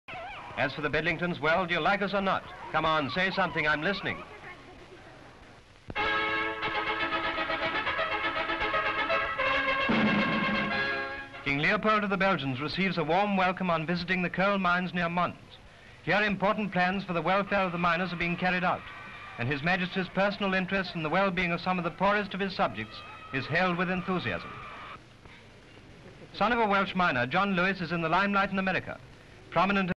King of the Belgians.
The King arrives at Coal Mines district. Crowds and children wave flags. Close shots of the King.